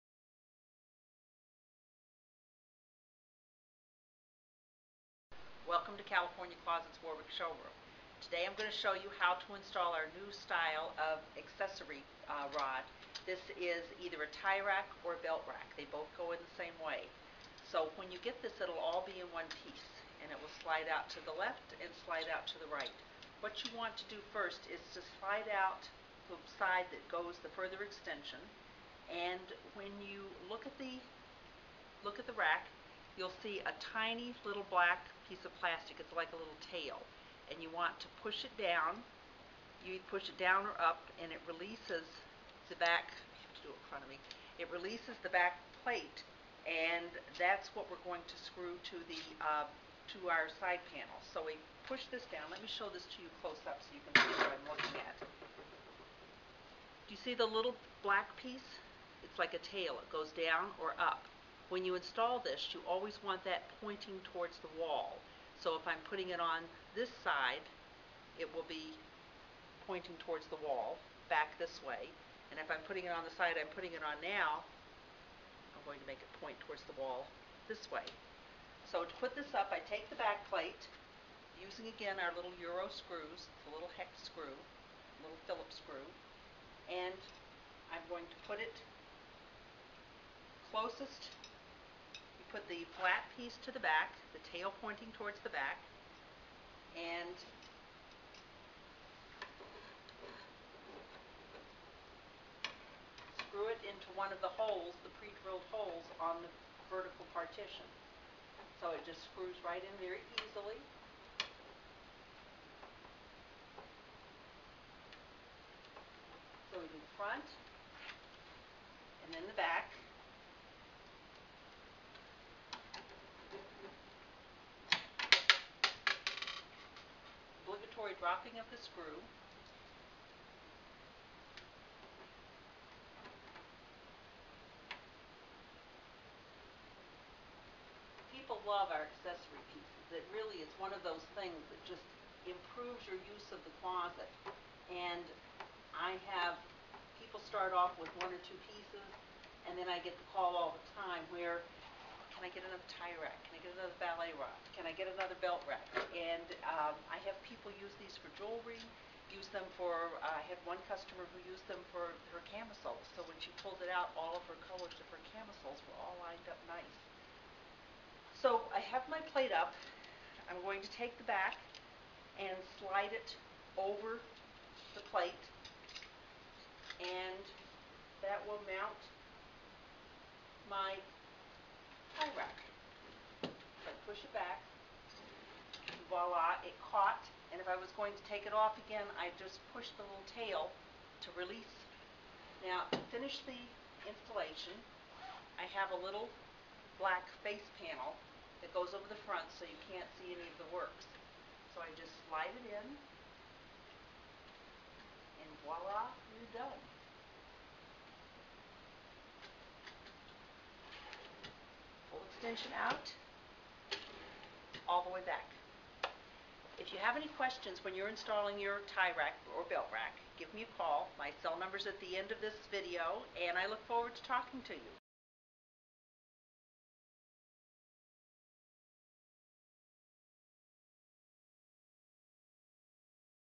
How to Install California Closets Accessory Rack in less than 5 min
Design Expert Mary Cool shows you how to install California Closets Accessory Rack in less than 5 minutes.  You can use it to organize belts, ties, even jewelry.